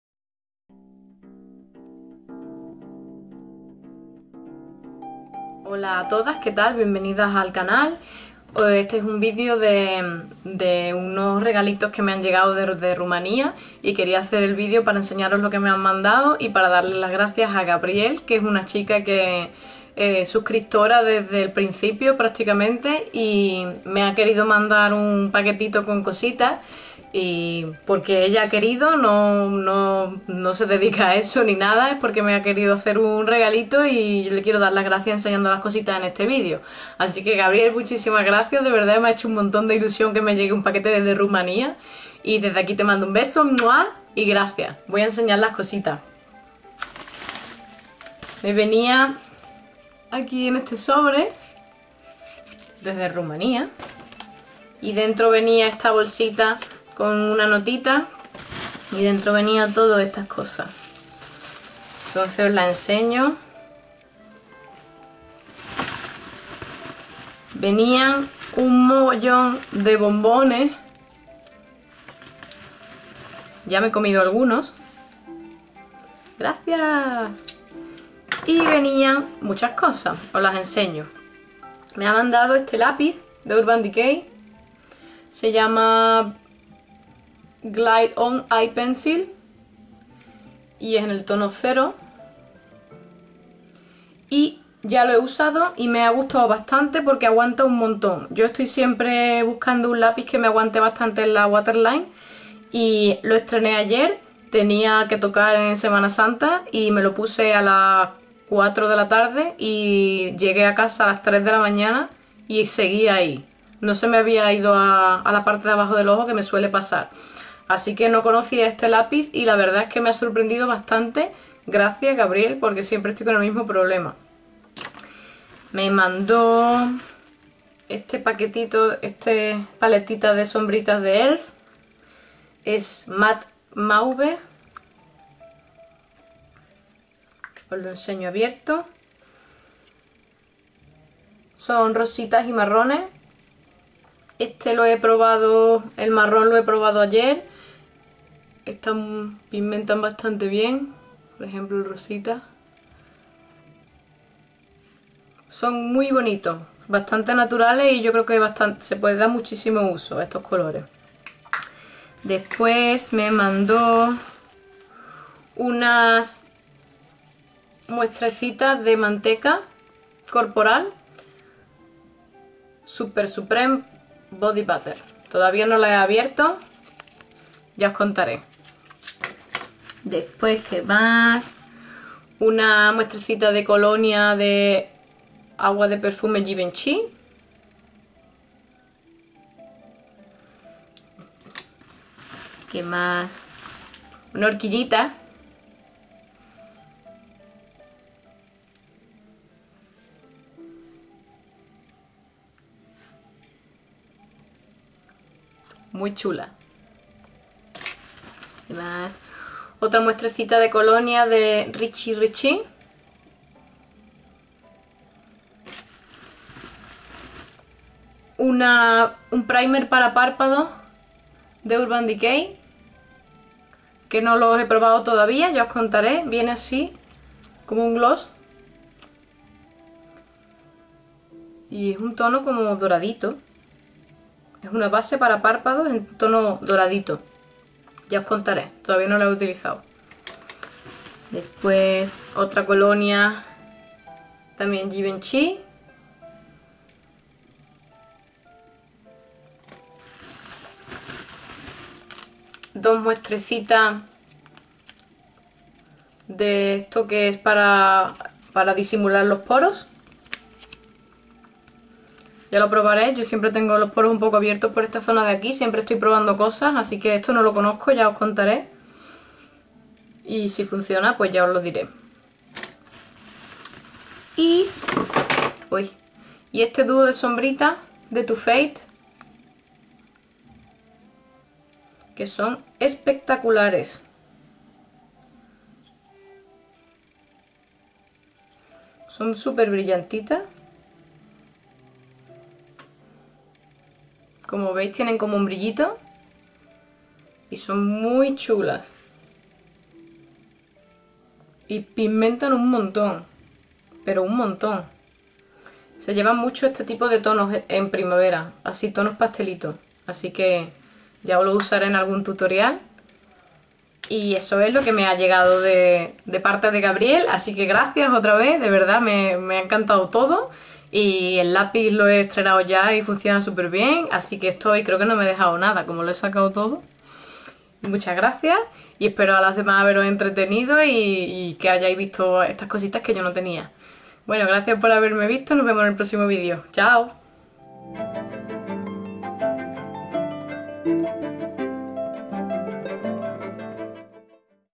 Haul de regalitos de Rumanía
Os enseño los regalitos que me ha mandado Gabriela, que es una de mis suscriptoras. Muchas gracias por el paquete guapa!